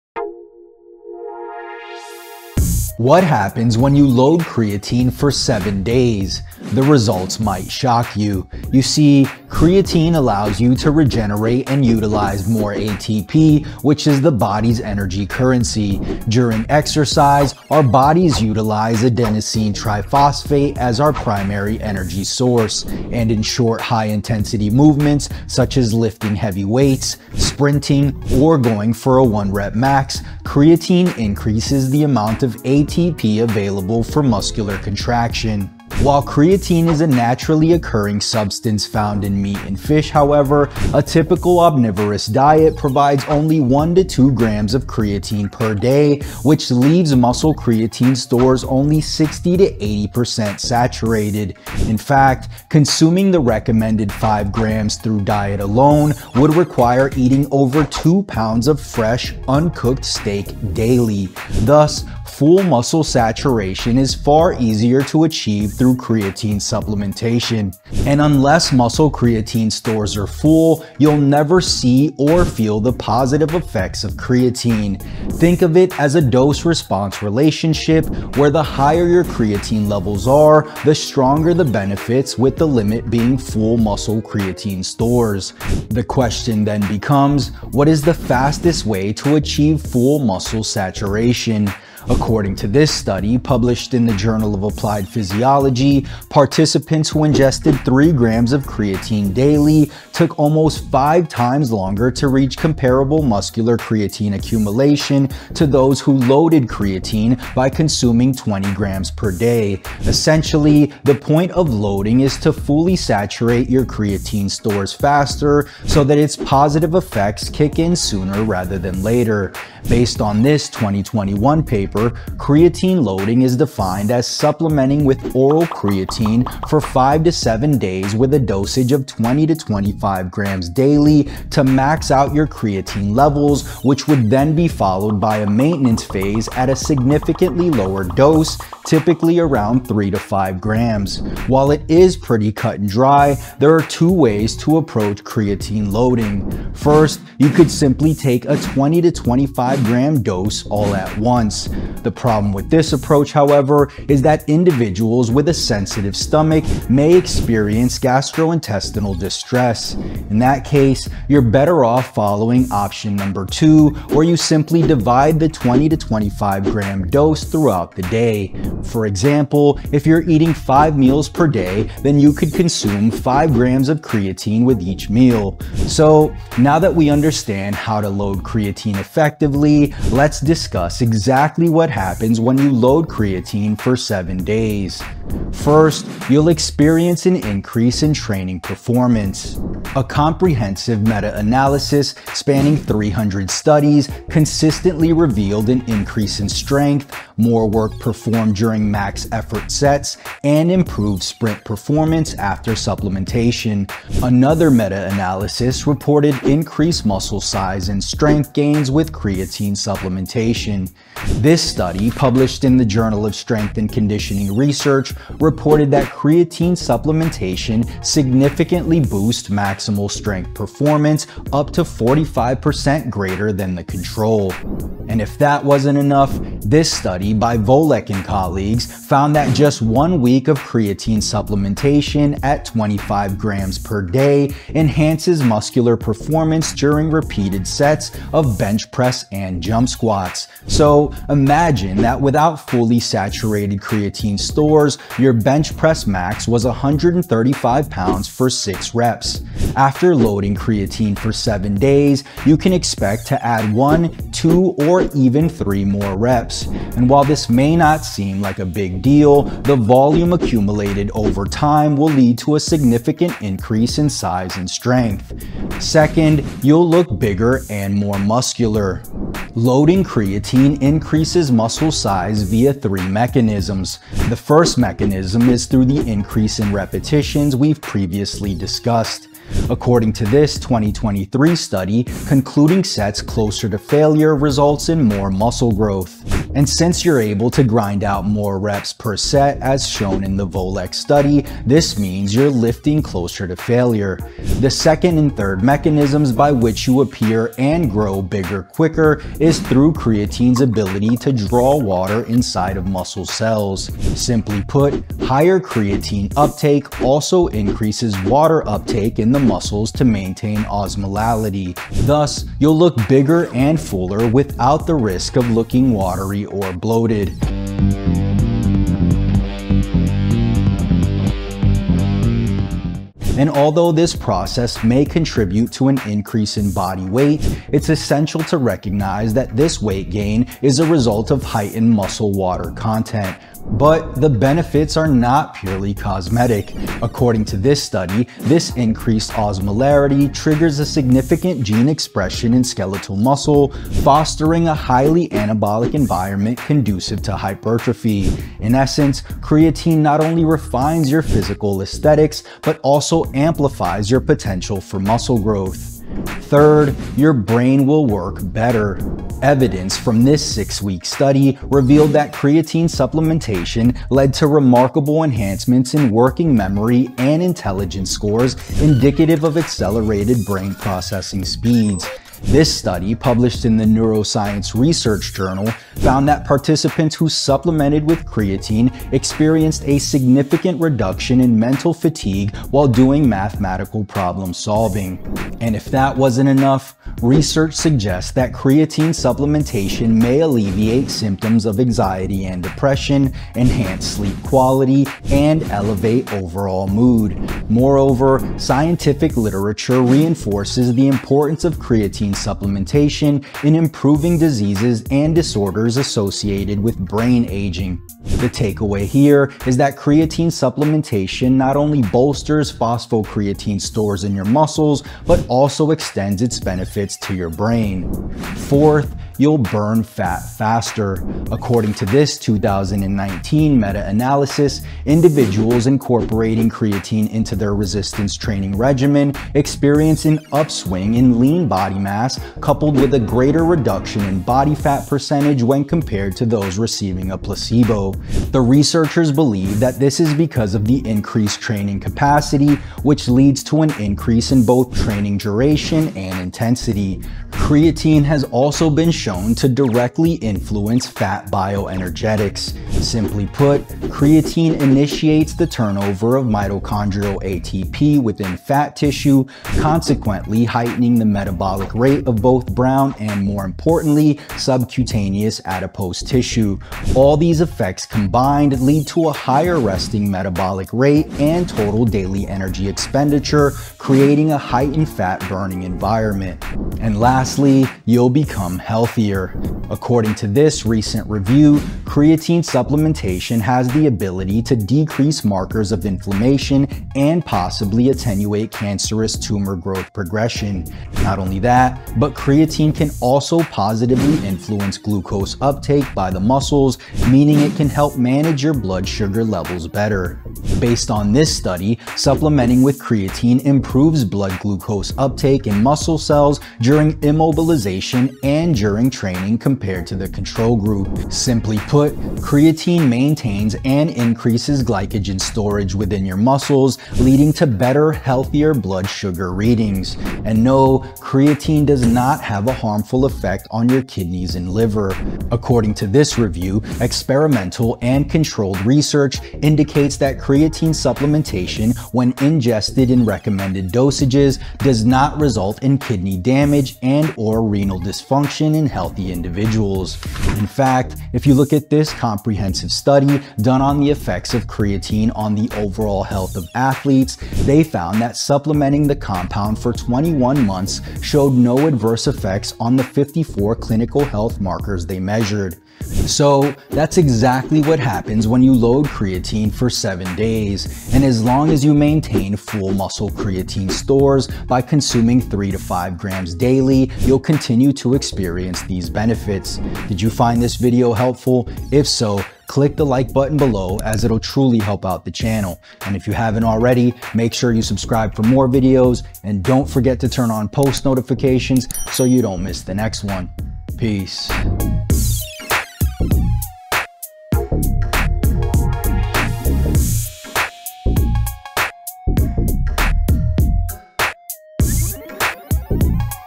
What Happens When You Load Creatine for 7 Days (science-based)
What happens to your body when you load creatine for 7 days?

In this video, I discuss 5 things you’ll experience once you’ve loaded creatine for 7 days.

And if you’re unsure about how to properly load creatine, I have you covered.

You'll also learn 2 simple strategies for loading creatine monohydrate so that you, too, can experience these results.

[SUPPLEMENTS] SCIENCE-BASED SUPPLEMENTS THAT WORK!